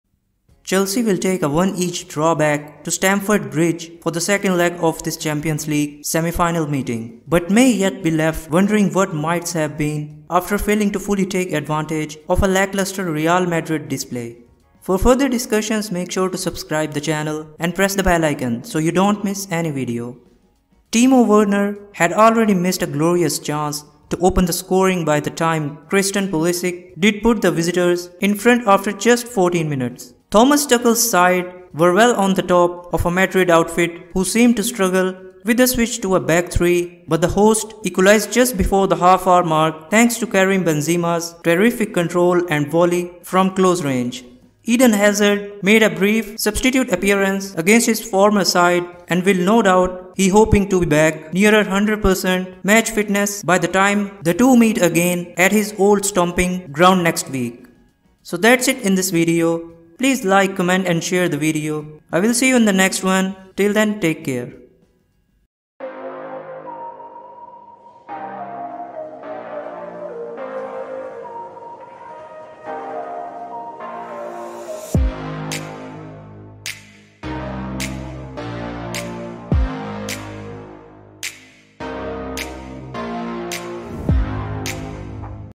Chelsea Vs. Real Madrid Semi Final || UEFA Champions League latest News And Reaction || Daily Trends
Chelsea will take a 1-1 drawback to Stamford Bridge for the second leg of this Champions League semi-final meeting, but may yet be left wondering what might’ve been after failing to fully take advantage of a lackluster Real Madrid display.
Timo Werner had already missed a glorious chance to open the scoring by the time Christian Pulisic did put the visitors in front after just 14 minutes.
Thomas Tuchel’s side was well on top of a Madrid outfit who seemed to struggle with the switch to a back-three, but the hosts equalized just before the half-hour mark thanks to Karim Benzema’s terrific control and volley from close range.
Eden Hazard made a brief substitute appearance against his former side and will no doubt be hoping to be back nearer 100 percent match fitness by the time the two meet again at his old stomping ground next week.